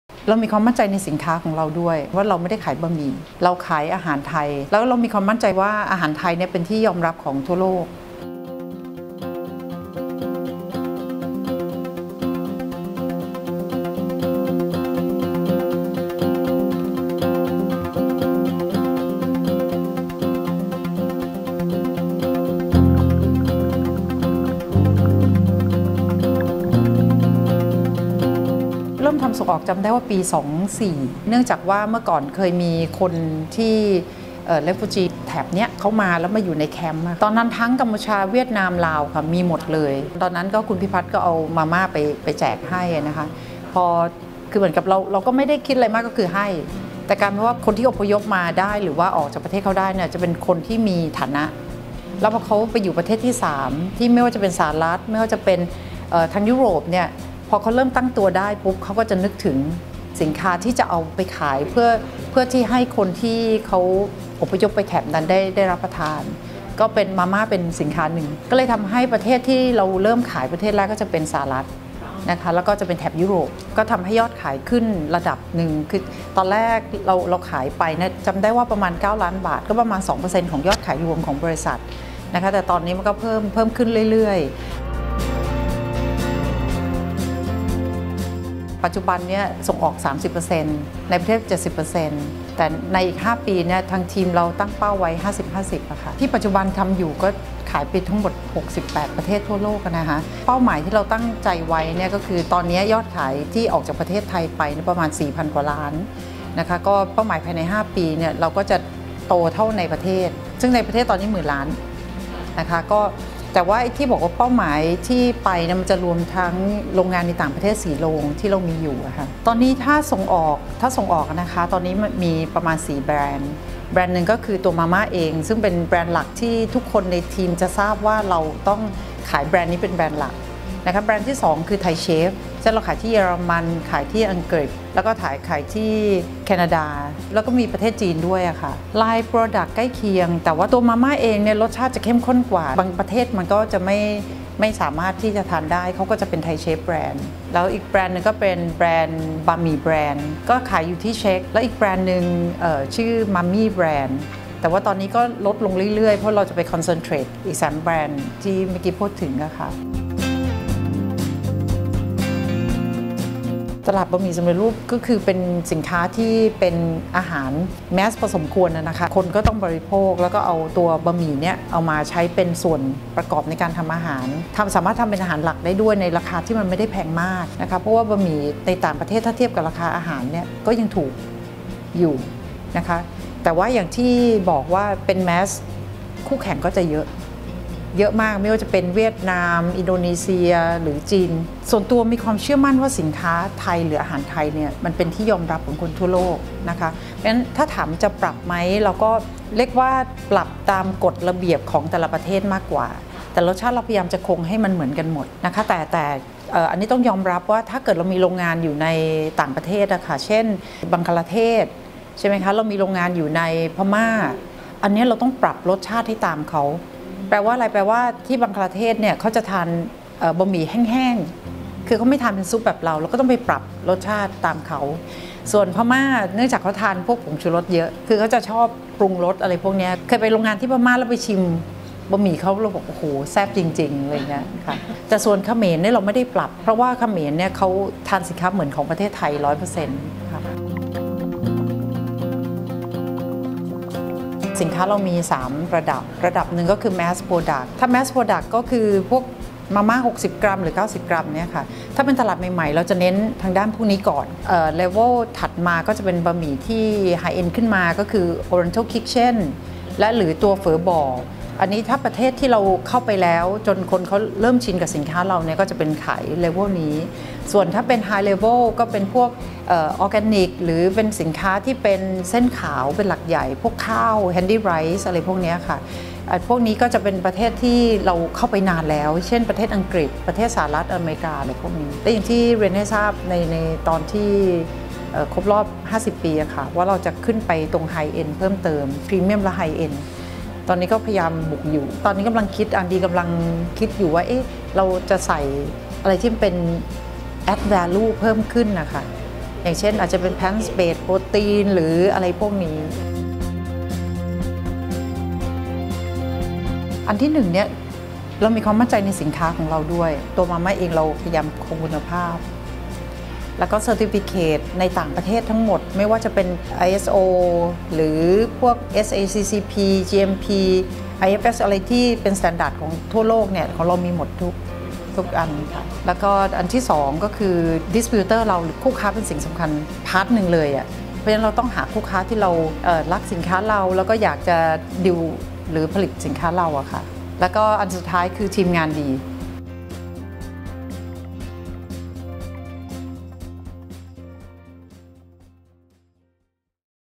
พจนา พะเนียงเวทย์ นำทัพ “มาม่า” เสิร์ฟลูกค้าทั่วโลก
Forbes Thailand พูดคุยกับ “พจนา พะเนียงเวทย์” ถึงอีกก้าวย่างสำคัญของผู้นำตลาดบะหมี่กึ่งสำเร็จรูปแบรนด์ไทย “มาม่า” ที่มองโอกาสกว้างไกลมากกว่า 68 ประเทศที่ทำตลาดอยู่ โดยตั้งเป้าว่าภายใน 5 ปี จะทำยอดส่งออกให้มีรายได้เท่ากับยอดขายในประเทศ ควบคู่ไปกับการสร้างสรรค์นวัตกรรมอาหารเสิร์ฟลูกค้าทั่วโลก
สามารถติดตาม Forbes Thailand Magazine ได้ทุกช่องทางดังนี้